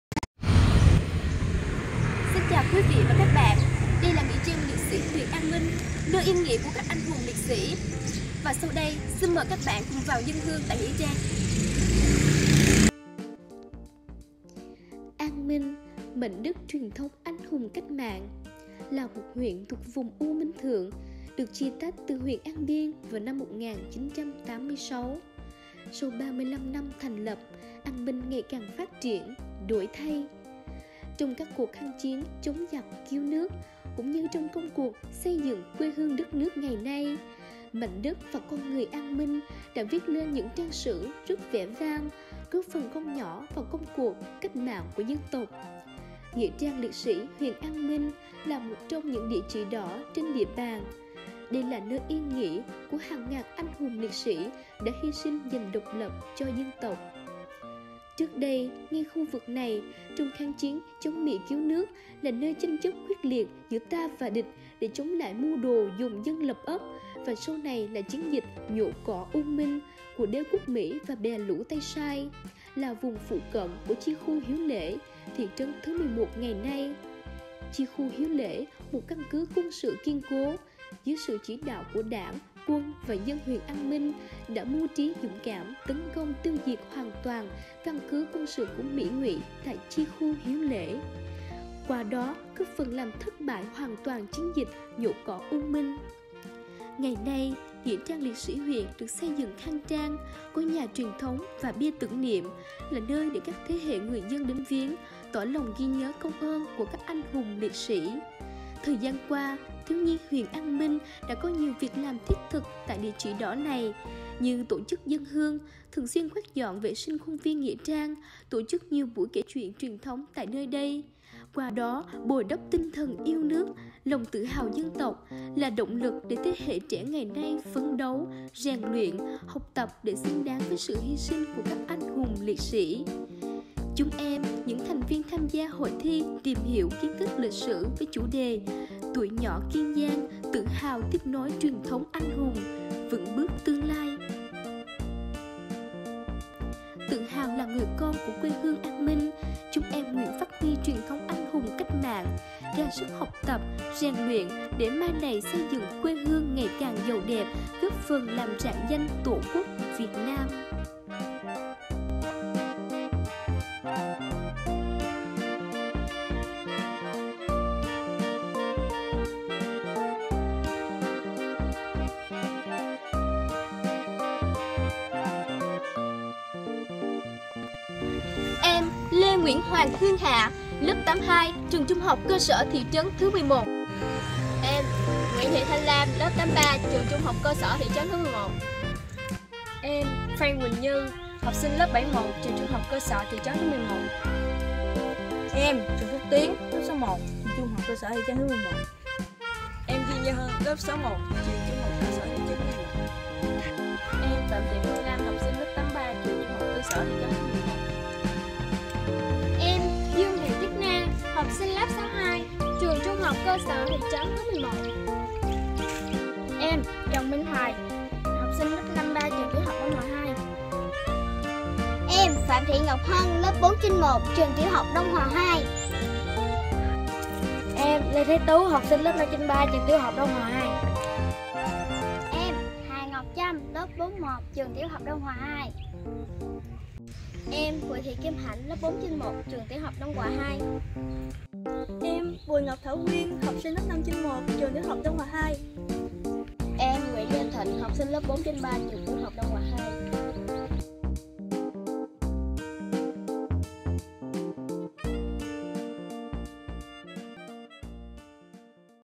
👉 THIẾU NHI MIỆTTHỨ❗➸ ❛HÀNH TRÌNH đến ĐỊA CHỈ ĐỎ❜ 📍 Nghĩa trang liệt sĩ huyện An Minh
👉 THIẾU NHI MIỆTTHỨ❗
      ❛HÀNH TRÌNH ĐẾN ĐỊA CHỈ ĐỎ❜‼️
               📍 Nghĩa trang liệt sĩ Huyện An Minh

      ❝ Sáng 18/5, Huyện Đoàn - Nhà Thiếu nhi
HuyệnAnMinh đã tổ chức ❛Hành trình đến
         địa chỉ đỏ❜ tại📍Nghĩa trang liệt sĩ Huyện
         An Minh với chủ đề ❛Kỷ niệm 80 năm ngày
         thành lập Đội TNTP Hồ Chí Minh ( 15/5/1941
- 15/5/2021). ❛Hànhtrìnhđếnđịachỉđỏ❜ có
sự tham gia của cán bộ NhàThiếunhi huyện
         và ➊➍ em học sinh là thành viên đội tuyển
         Huyện An Minh cấp THCS và Tiểu học sẽ
         tham gia Vòng chung kết Hội thi tìm hiểu
kiến thức với chủ đề ❛Tuổi nhỏ
         tự hào tiếp nối truyền thống anh hùng,
         vững bước tương lai❜ tại Nhà Thiếu nhi
        Tỉnh Kiên Giang vào tháng ➏ tới❞

        Cùng xem 📺 thú vị này nhé‼️❜

               📽️  AkiQuang